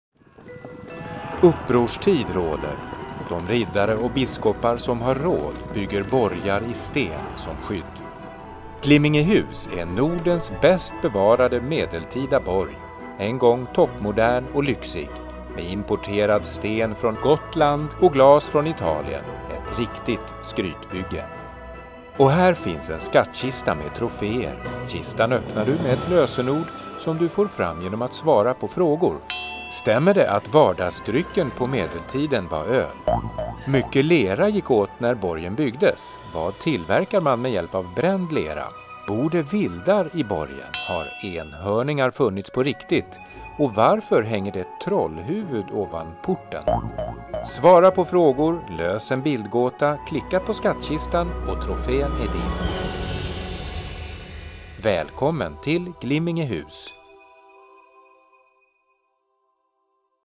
Glimmingehus
Filmen berättar om en upprorstid, en väl bevarad medeltida borg och enhörningar.